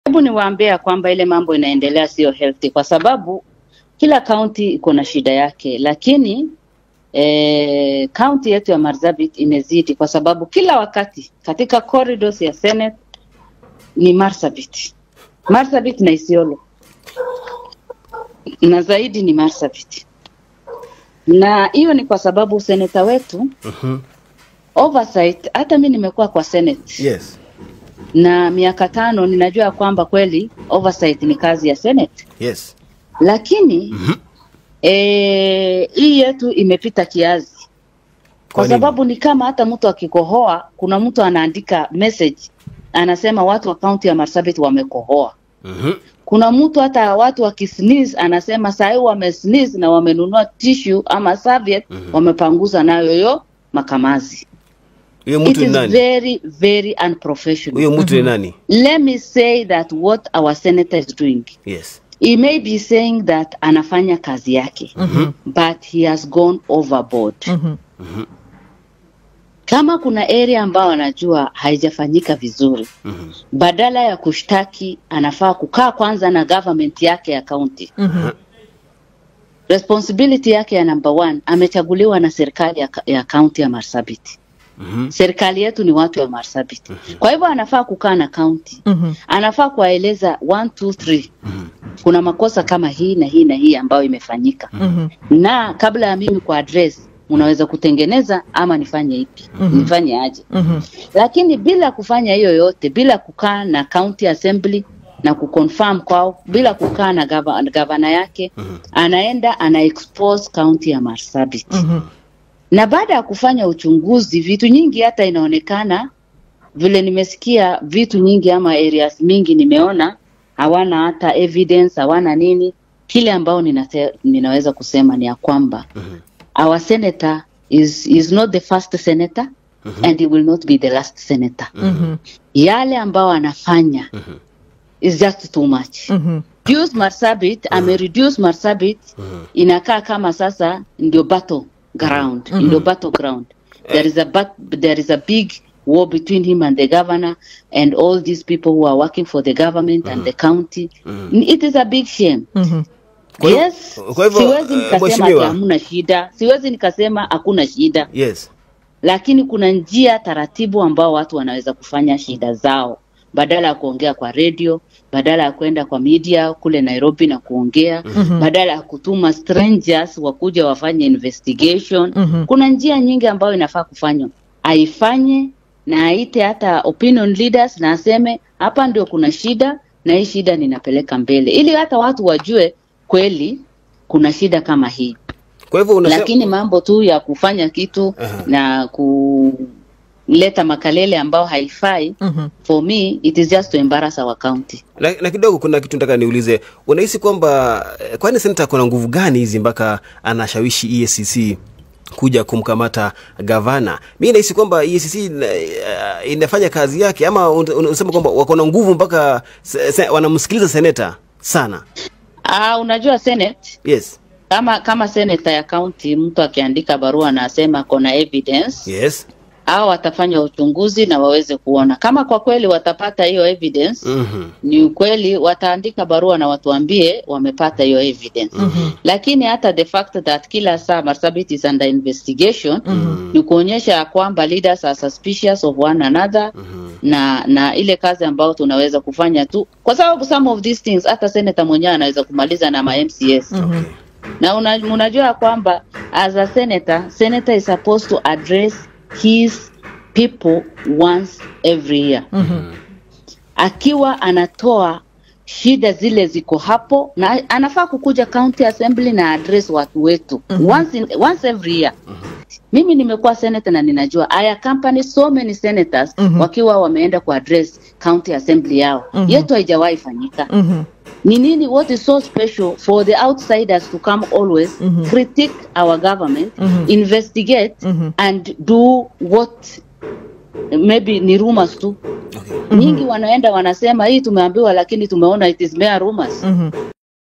NAOMI WAKO ATTACKS Senator CHUTE for BEING NUISSANCE TO MARSABIT GOVERNOR MOHAMMUD ALI
Culex Media Africa